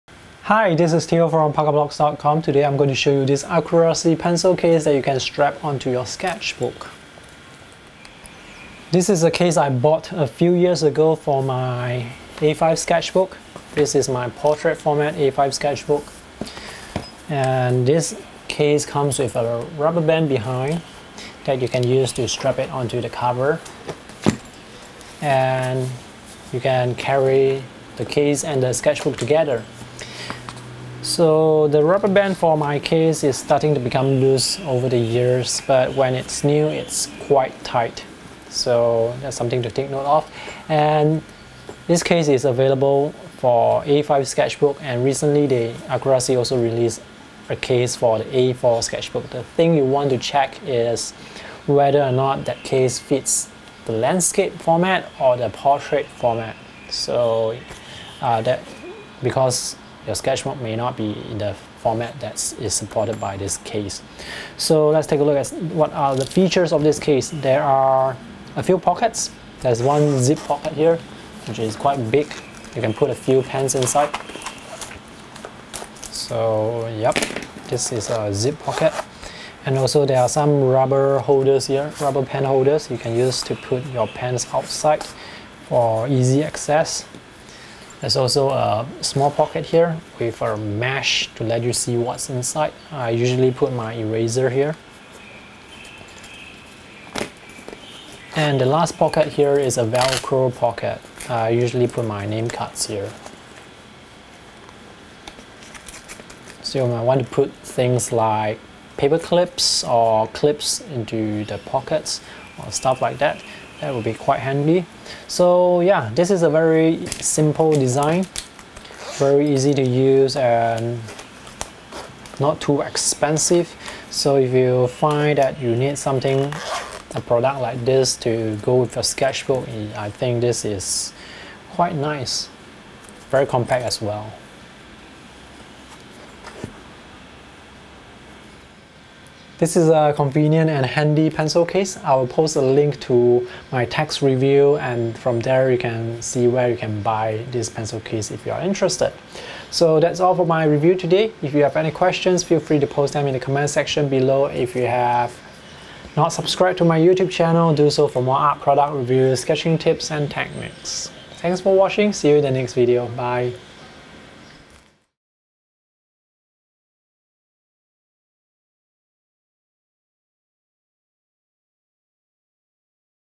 Review: Accurasee Pencil Case (Sketch Caddie)
The Accurasee pencil case comes in two sizes for A5 and A4 sketchbooks.